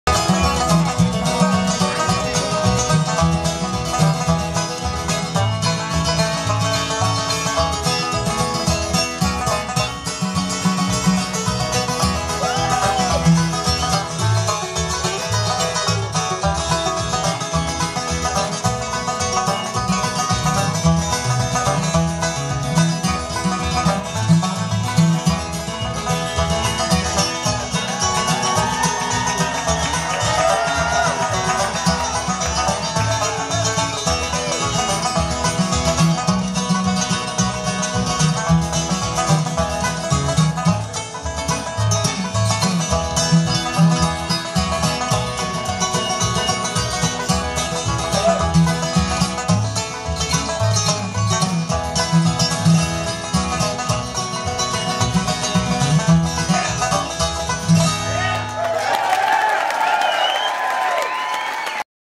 John Rigsby Pt 2
John Receiving A Carter Stanley Memorial guitar for being a Clinch Mnt Boy @ Ralph Stanley 46th Hills Of Home Festival...On Stage With Him Are The Children Of Carter Stanley And The Son Of Ralph Stanley..Ralph Stanley II.